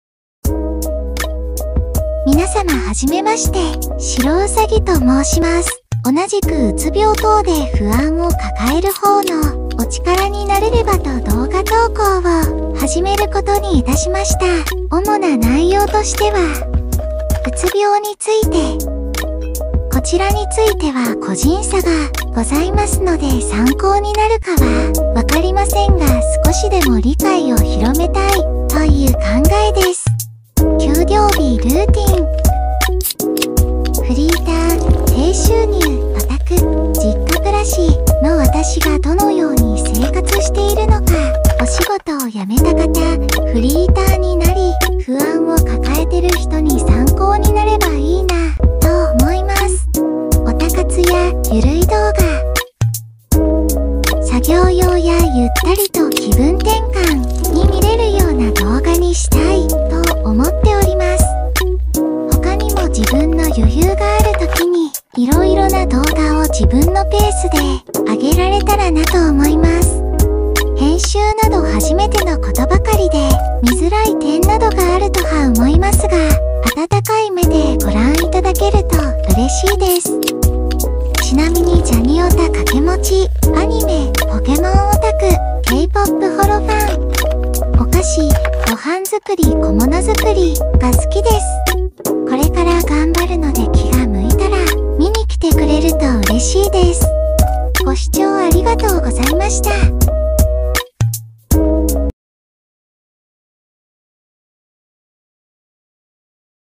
［初投稿自己紹介］躁鬱20代前半のお話
動画を閲覧ありがとうございます♪
自己満での動画投稿になりますので暇つぶしにでも見ていただけると嬉しいです！
躁鬱のため投稿頻度はまちまちですが1人でも多くの不安を抱える方の応援ができればと思います🤍
今回のみ音声ありです📣